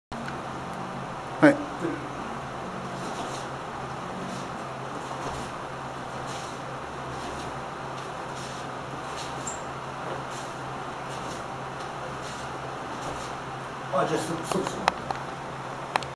Hiko Ryu Taijutsu Basic Training 1-1 Kiyoshi Takeuchi
Hiko Ryu Taijutsu was taught to the Narcotics Surpression Bureau Royal Thai Police, the Philippine National Police SWAT, the Dalai Lama's security guards and others. It is the realistic combat self-defense style.

Kiyoshi Takeuchi